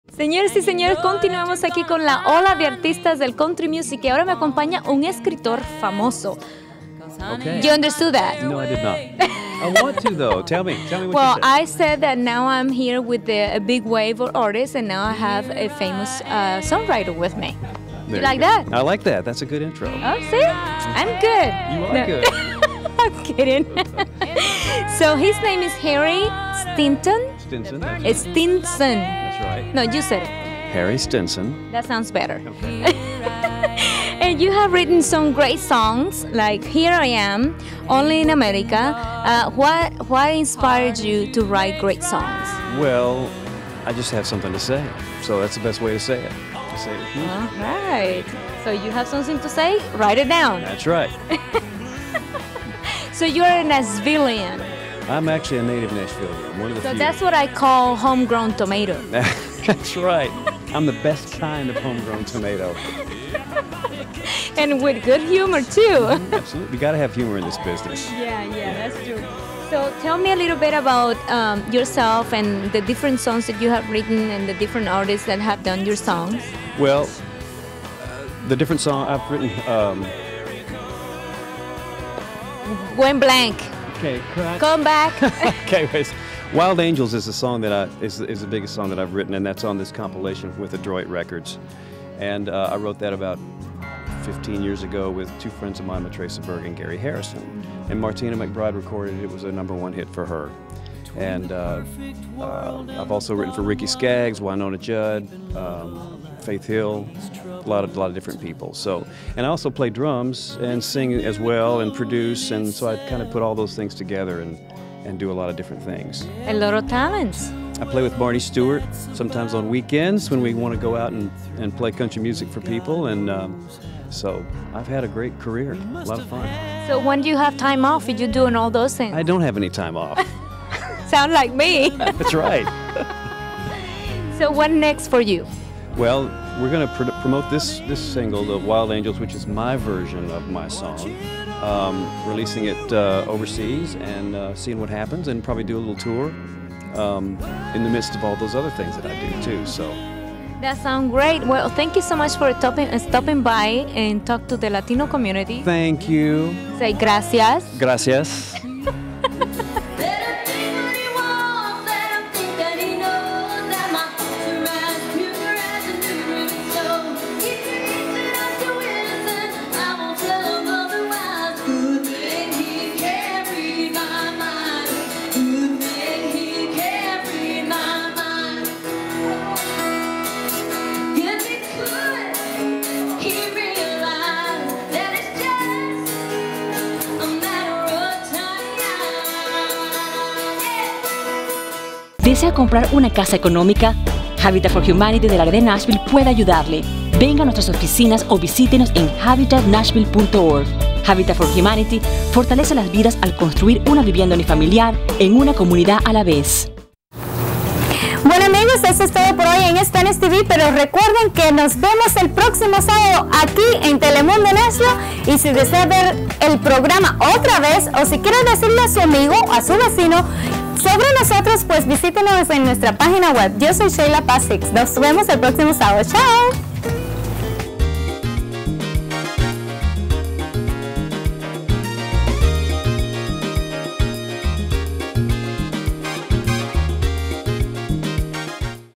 SPANISH-TV S9 # 114 Week 11 Interview to Harry Stinson.mov
Sheyla Paz Hicks interviews successful songwriter Harry Stinson during the CMA Fest in Nashville